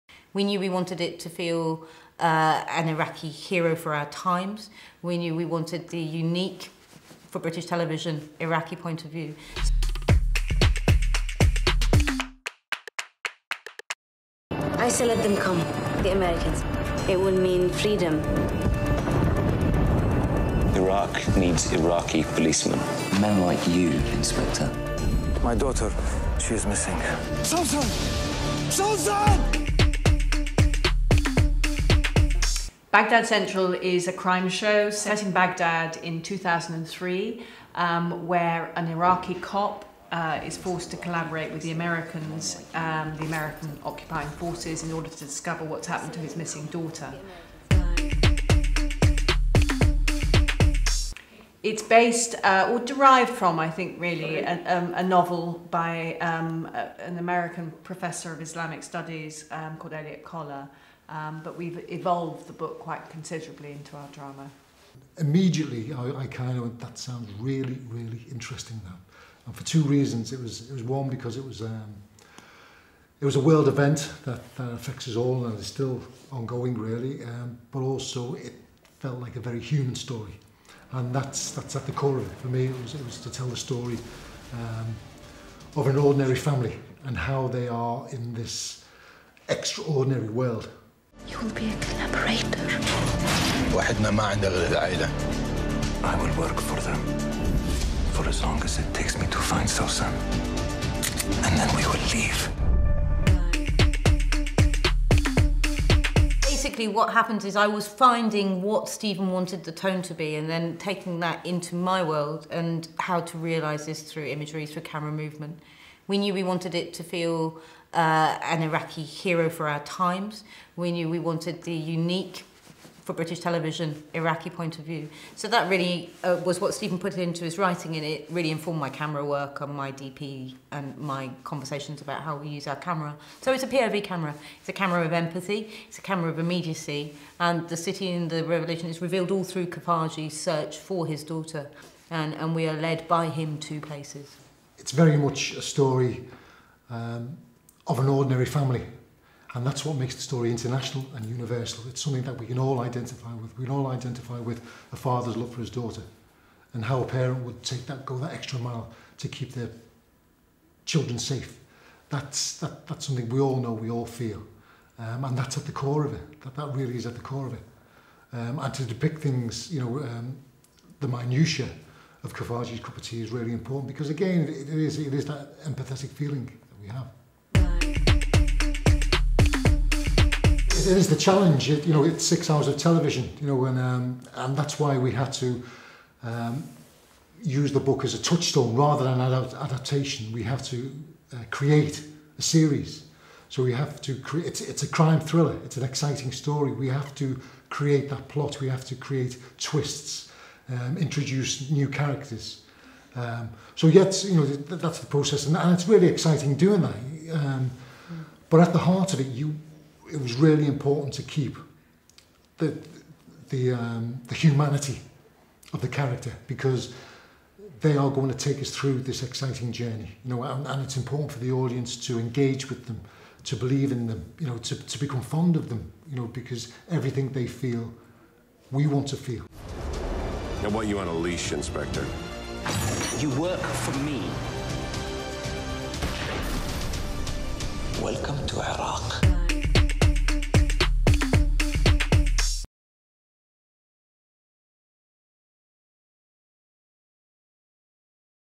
Baghdad Central - à voir sur Arte
🔔 Et n'oubliez pas d'activer vos notifications 🔔

A l'occasion de la sortie sur Arte de Baghdad Central le 1er avril 2021, découvrez notre interview de l'équipe à l'origine de la série, sélectionnée en Compétition internationale à Séries Mania 2019 :
Alice Troughton : réalisatrice et productrice exécutif
Stephen Butchard : scénariste et producteur exécutif
Kate Harwood : productrice exécutive

A voir à partir du 01/04/2021 sur Arte.tv et sur Arte à partir du 08/04/2021

Synopsis :
Irak, 2003. À la suite de la chute de Saddam Hussein, Baghdad est plongée dans le chaos. Les forces de sécurité locales sont démantelées par la coalition internationale, alors même que les insurgés politiques poursuivent le combat. Américains et Britanniques tentent de sécuriser le pays et Muhsin al-Khafaji, un ex-policier, est contraint de travailler avec eux pour identifier parmi les siens qui collaborera. Lorsque la fille de Muhsin disparaît, une course contre la montre est engagée.
Baghdad Central est basé sur une nouvelle d'Elliot Colla. La série a été créée et écrite par le scénariste Stephen Butchard (The Last Kingdom, House of Saddam), nommé aux BAFTA.